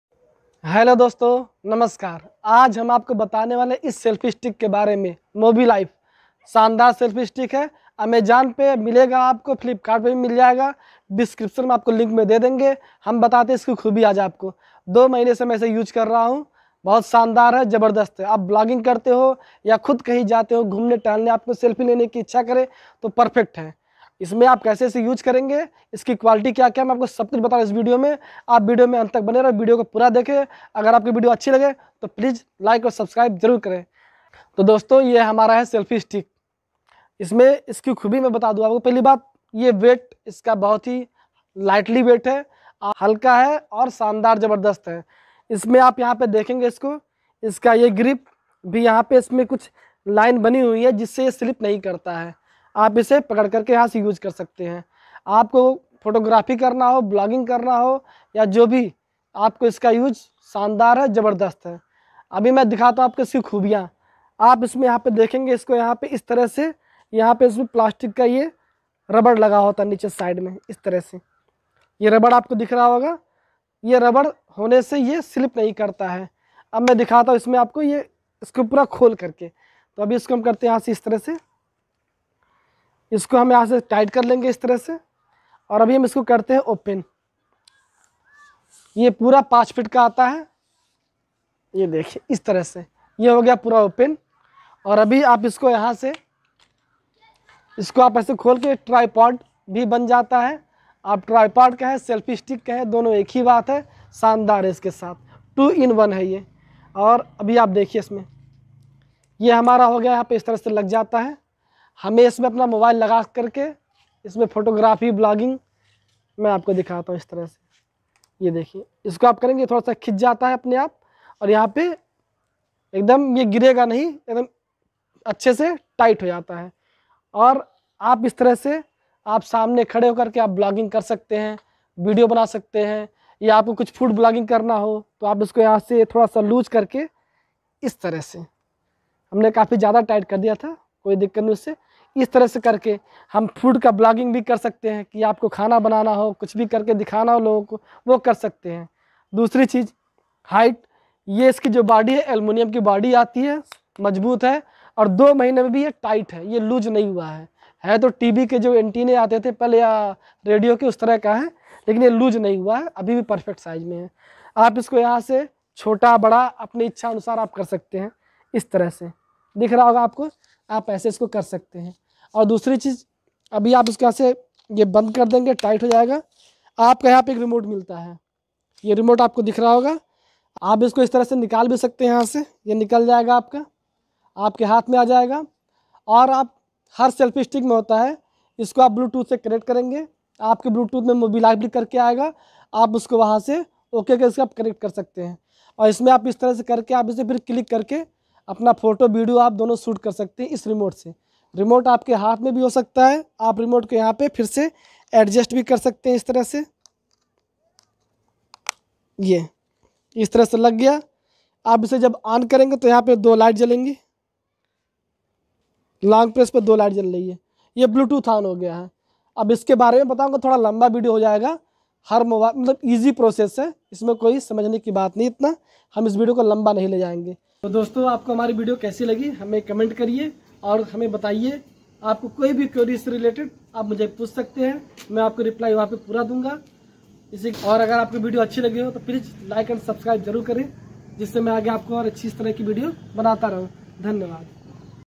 Mobilife Selfie Stick Tripod || Longest selfie stick
hello friends 😁👋

यह एक अच्छा और बजट में आने वाला product हैं इस दाम का ये सबसे अच्छा product हैं
मैने इसे 1700 में लिया था आपको एक अच्छी बात बताए कई tripod में ऊपर के हिस्से में मोबाइल जहा पे सेट करते है वो हिलता है मगर ये अच्छा और मजबूत
product हैं |

buy this product link--

Rahul Srivastav
Nautanwa ( U P )